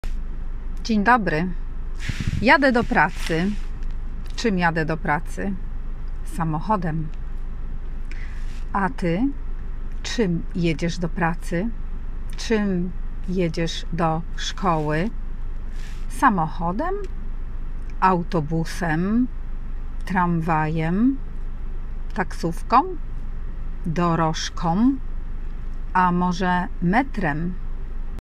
Karta Polaka nauka polskiego w minutę! Czym jedziesz do pracy? odc. 14
Ucz się polskiego z Kartą Polaka codziennie 1 minutę! Odcinek 14.
Czym jedziesz do pracy / do szkoły?
Jadę do pracy:
rodzaj męski: jadę samochodEM, autobusEM, tramwajEM - em
rodzaj żeński: taksówkĄ, dorożkĄ - ą
rodzaj nijaki: metrEM - em